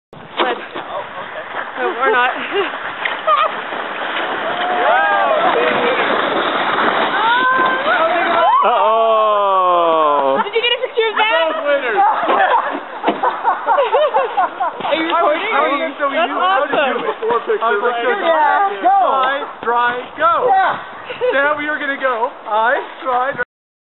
Adam and Andrew Sledding Race 12/30/09
Andrew and Adam race sleds.  Andrew makes it down the mountain first, but who wins?